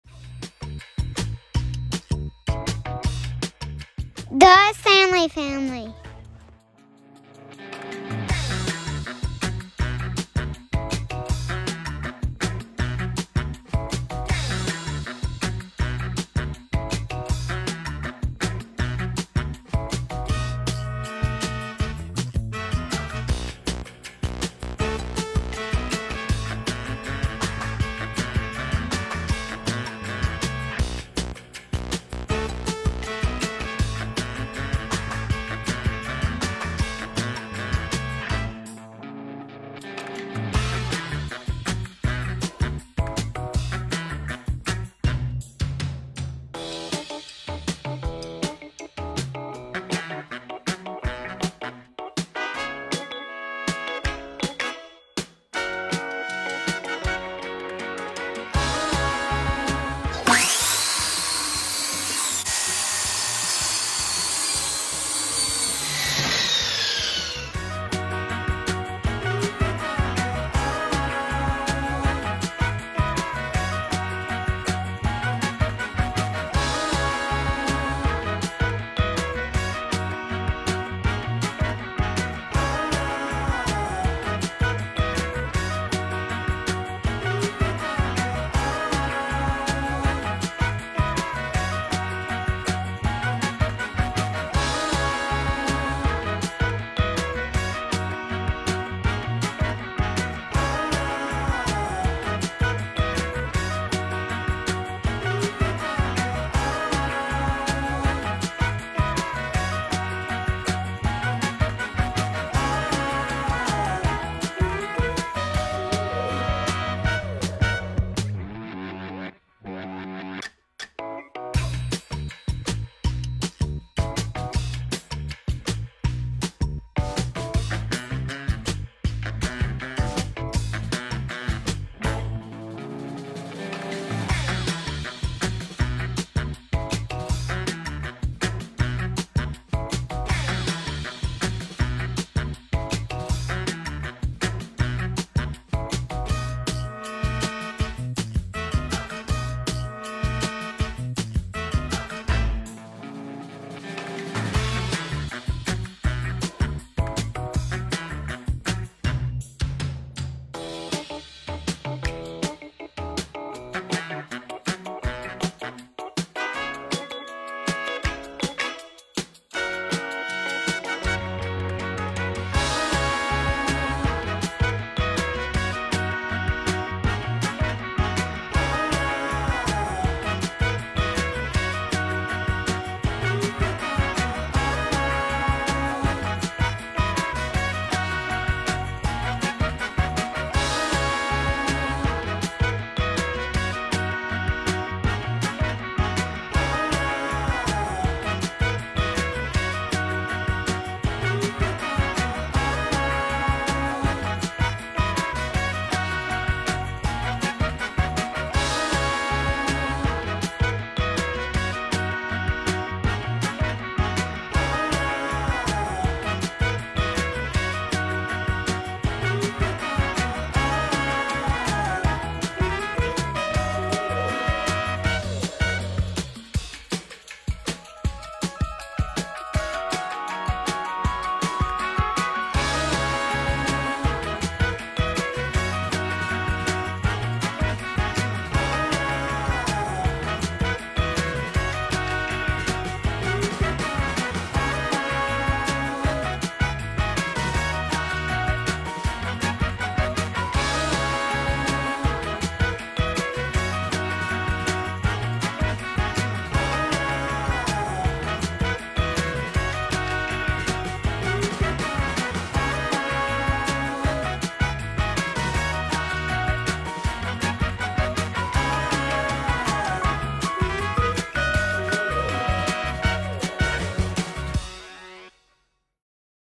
Kitchen Remodel / House Flip/ Kitchen Renovation
Our kitchen got a much needed upgrade! We did beadboard ceilings, painted the cabinets, bought new hardware, made a DIY brick backsplash, wrapped the bar with beadboard, and upgraded the flooring.